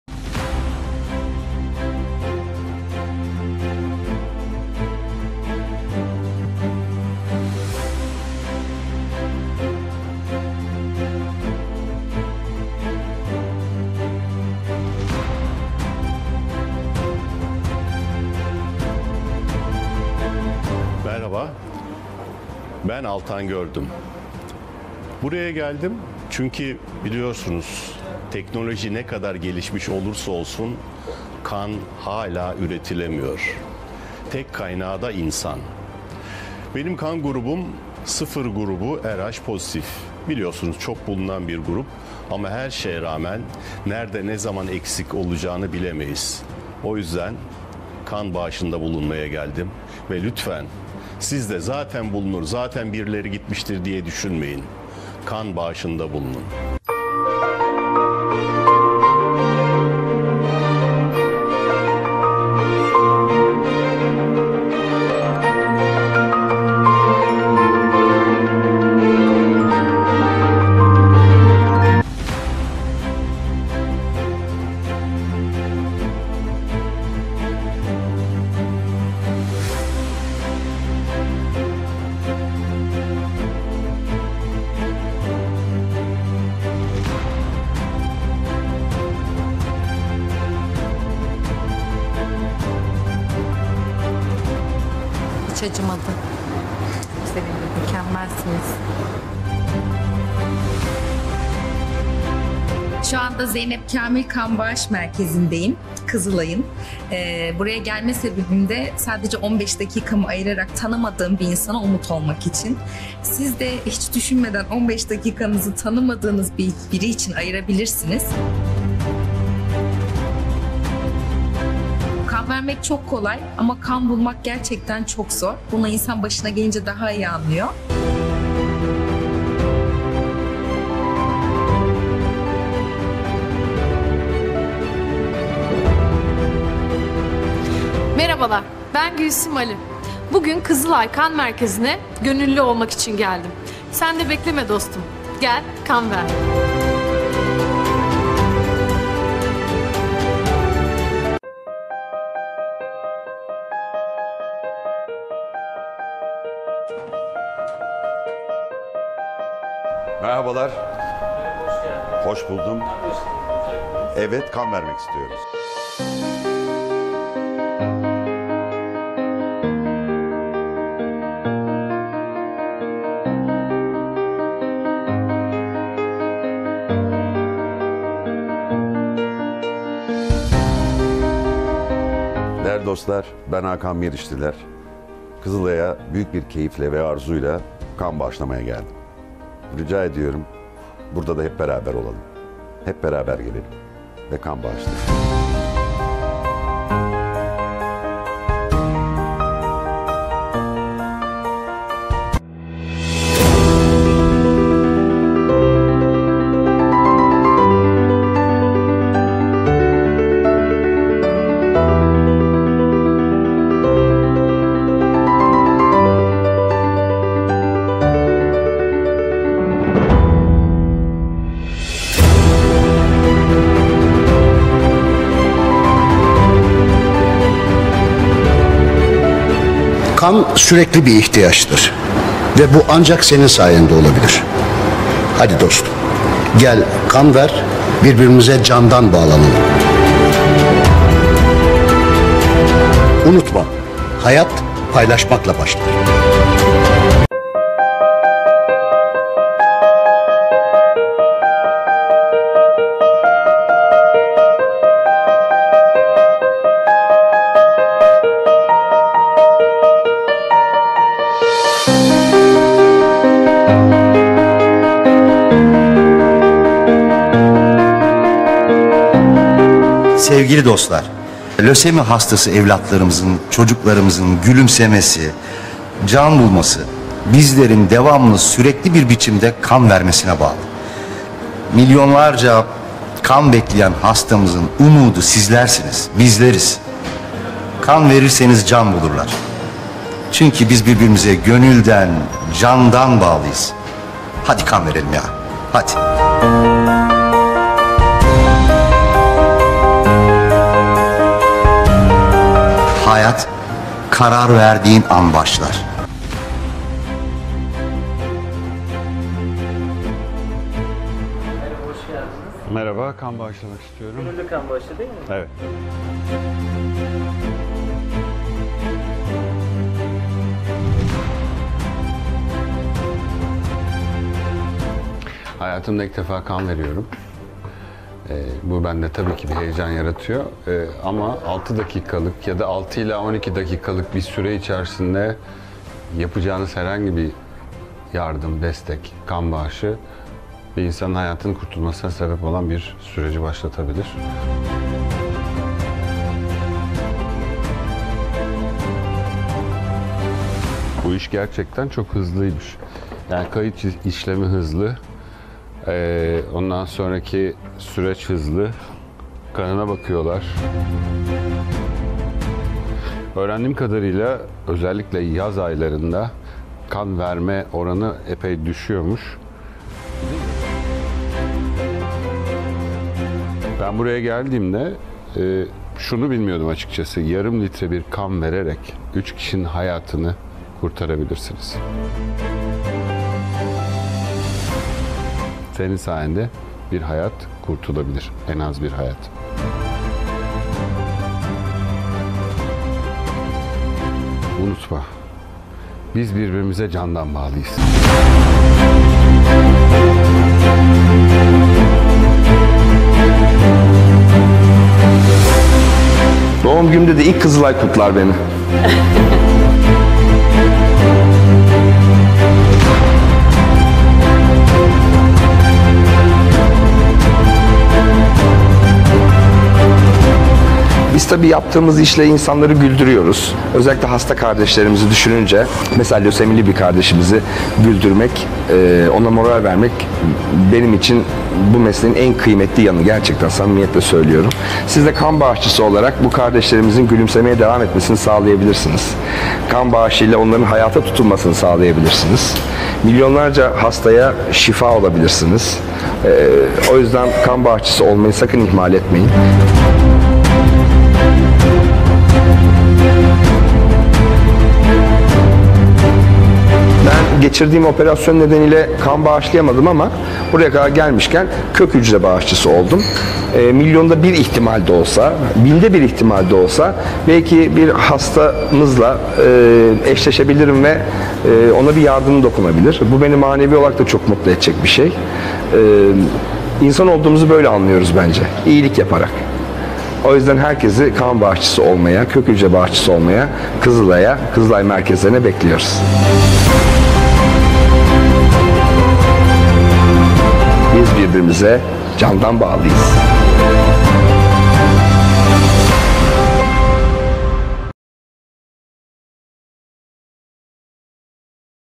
Ünlü oyuncular Türk Kızılay'a kan bağışı çağrısı yaptı(GÖRÜNTÜ EKLENDİ)
İSTANBUL,(DHA) - Aralarında Didem Balçın, Gülsim Ali, Murat Aygen, İlker Aksum, Ceren Benderlioğlu'nun da yer aldığı ünlü oyuncular, yaz aylarında mevsimsel olarak düşen kan bağışları nedeniyle azalan stok seviyesine dikkat çekmek üzere kan bağışında bulunarak Türk Kızılay'ın yürüttüğü 'Birbirimize Candan Bağlıyız' kampanyasına destek çağrısı yaptı.\r\n\r\nSağlık Bakanlığı tarafından verilen kan hizmetleri görevi çerçevesinde 18 Bölge Kan Merkezi, 69 Kan Bağış Merkezi, 300'den fazla mobil ve sabit ekip ile kan bağışı çalışmalarını yürüten Türk Kızılay, ülkemizin kan ihtiyacının tamamını gönüllü bağışçılardan sağlamak üzere 'Birbirimize Candan Bağlıyız' kampanyasını yürütüyor.
 \r\n\r\n2024 yılının ilk 6 ayında Türkiye'nin kan ihtiyacının yüzde 96'sını karşılamayı başaran Kızılay, yaz aylarında mevsimsel olarak düşen kan bağışına bağlı olarak azalan stok seviyesini artırmak üzere çalışmalar yürütüyor.
 Bu kapsamda ünlü oyuncular Didem Balçın, Gülsim Ali, Murat Aygen, İlker Aksum, Ceren \u0026 Emir Benderlioğlu, Altan Gördüm, Hakan Meriçliler, Hüseyin Avni Danyal ve Rüştü Onur Atilla, kan bağışında bulunarak 'Birbirimize Candan Bağlıyız' kampanyasına destek verdi.
 Ünlü oyuncular, yaz aylarında azalan stoklara ve kanın sürekli bir ihtiyaç olduğuna vurgu yaparak 18-65 yaş arası sağlıklı her bireye düzenli kan bağışçısı olma çağrısı yaptı. \r\n\r\n“KAN VERMEK ÇOK KOLAY, BULMAK ÇOK ZOR\"\r\nKızılay Kan Merkezi'nde kan bağışında bulunan oyuncu Didem Balçın, “Kan vermek çok kolay ama kan bulmak gerçekten çok zor.
 Bunu insan başına gelince daha iyi anlıyor. Benim de böyle bir deneyimim oldu bundan 2-3 sene önce. Ve o zaman öğrendim ki aslında bütün kanlar Kızılay'dan hastanelere yollanıyor. Kızılay'a bağış yapmak işte bu yüzden çok önemli\" dedi.\r\n\r\n“TEKNOLOJİ NE KADAR GELİŞSE DE KANIN TEK KAYNAĞI İNSAN\"\r\nOyuncu Altan Gördüm ise “Buraya geldim, çünkü biliyorsunuz teknoloji ne kadar gelişmiş olursa olsun kan hâlâ üretilemiyor.
 Tek kaynağı da insan. Buraya kan bağışında bulunmaya geldim, lütfen siz de 'Zaten bulunur' diye düşünmeyin, kan bağışında bulunun\" diye konuştu.
 \r\n\r\n“YAZ TATİLİNE ÇIKMADAN YAPIN\"\r\nEmir ve Ceren Benderlioğlu çifti de “Yaz aylarında kan verme oranları ciddi oranda düşüyor.
 Bu, pek çok hasta için çok ciddi sorun teşkil ediyor. Haydi sen de en yakınındaki Kızılay Kan Merkezi'ne git ve bağışını yap. Yaz tatiline çıkmadan bunu yapman çok önemli\" ifadelerini kullandı. \r\n\r\n“ÜÇ KİŞİNİN HAYATINI KURTARABİLİRSİNİZ\"\r\nOyuncu Murat Aygen ise “Yarım litre kan vererek 3 kişinin hayatını kurtarabilirsiniz.
 Senin sayende bir hayat kurtulabilir. En az bir hayat. Unutma! Biz birbirimize candan bağlıyız\" dedi. \r\n\r\n